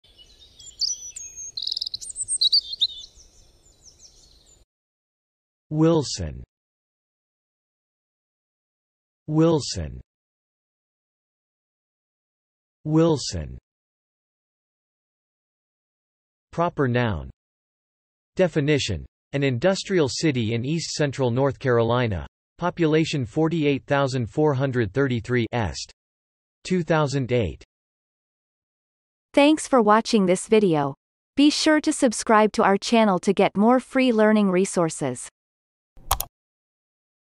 How to pronounce wilson - Vocab Today
The pronunciation of the word wilson in Amercian accent is demonstrated in this video. If the word is from another language, such as brand name, it will be pronounced in its original language. Based on the word frequency, this is of 2 stars difficulty.

The word wilson has the following meanings:
1. (proper noun)
Definition : An industrial city in east central North Carolina; population 48,433 (est. 2008).